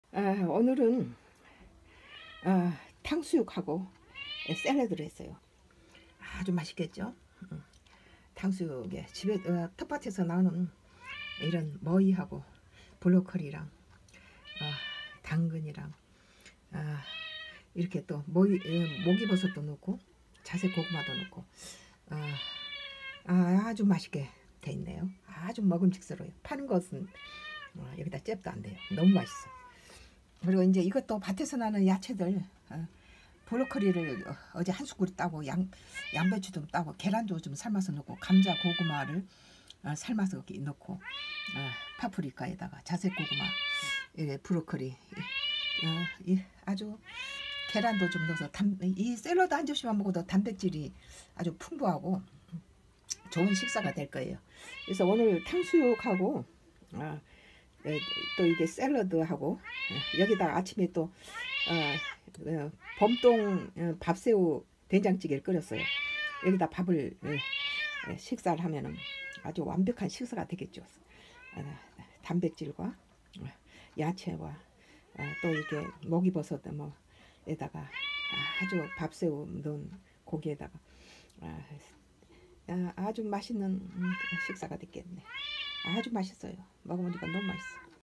MAMA FOOD 엄마표! 오늘 점심 메뉴@sungodcross
엄마표! 봄동 밥새우 된장국 !

오늘 아침식사엔~~~봄동 밥새우 된장국을 참치액젖 넣고 끓였는데
온식구가 넘 맛있다고하여 점심 때도 밥새우 봄동 된장국을 먹으려고~~~!
남은 국에 봄동과 밥새우를 더넣고~~된장을 한숟깔 풀어서~~끓이고 있다~~!

오늘 점심엔~~~어제 밭에서 뽑아온 당근,브로커리,양배추에~~고구마,감자,계란삶아서
쌜러드를 만들어서~~~탕수육과 함께 점심을 하려고 한다~~~!@sungodcross